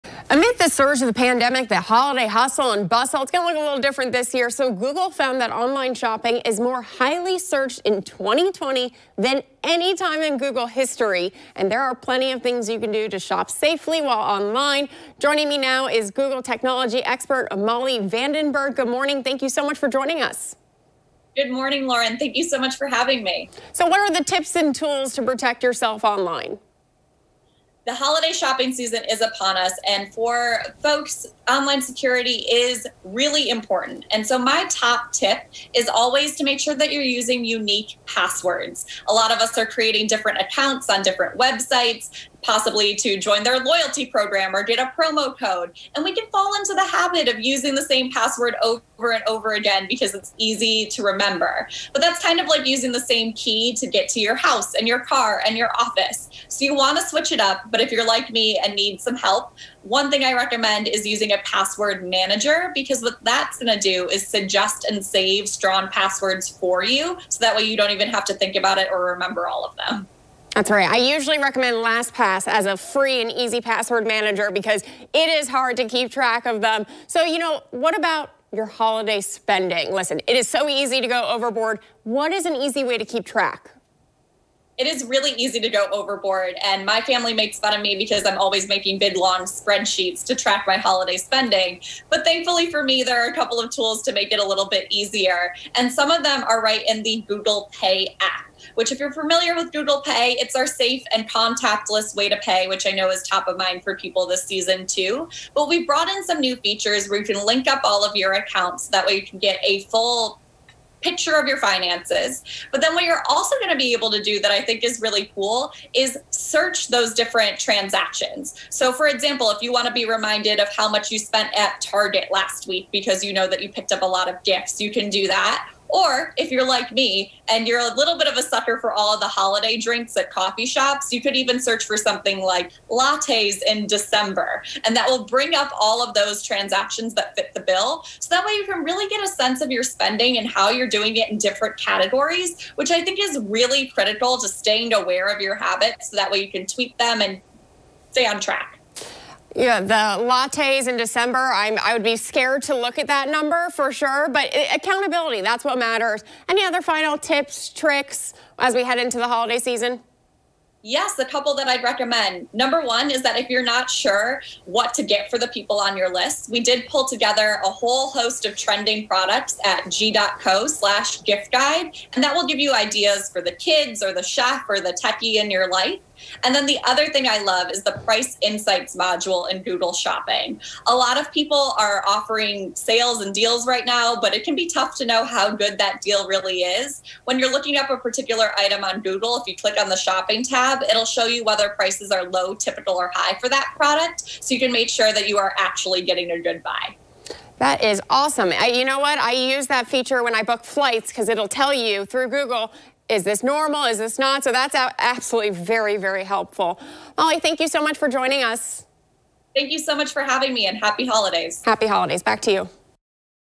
Shopping Online Safely
As more people choose to holiday shop online, it is important to protect yourself. Google Technology Expert, Molly Vandenberg joins us to give us some tools to protect ourselves when we shop online.